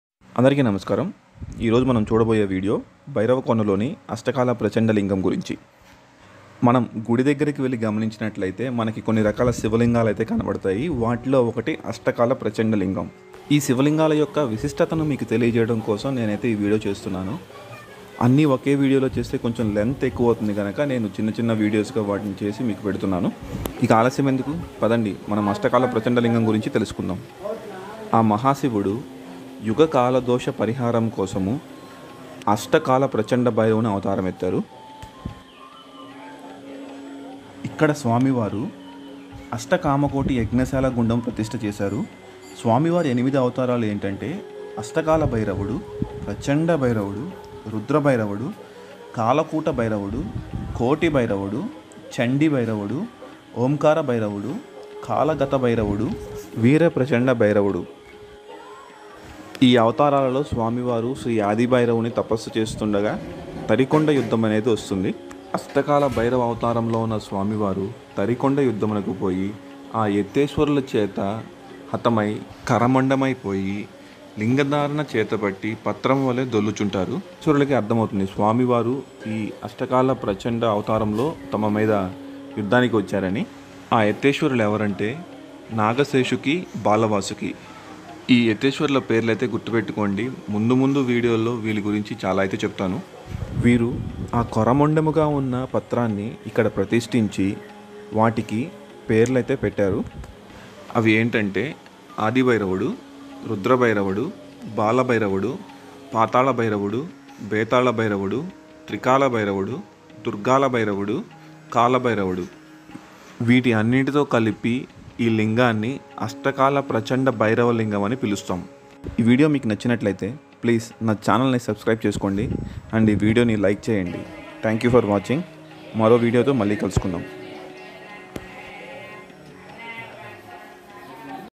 అష్ట కాల ప్రచండ భైరవ లింగం, భైరవ కోన || significance of asta kala lingam at bhairava kona
ఈ అద్భుతమైన లింగం భైరవ కోన లో ఉంది. ఈ లింగానికి ఆ పేరు ఎలా వచింది అన్న విషయం నేను గుడి పుస్తకం ద్వారా తెలుసుకున్నాను.   ఈ గుడి అద్భుతాలు గురించి ముందు ముందు వచ్చే వీడియోలో తెలియచేస్తాను

అందరూ బాగుండాలి.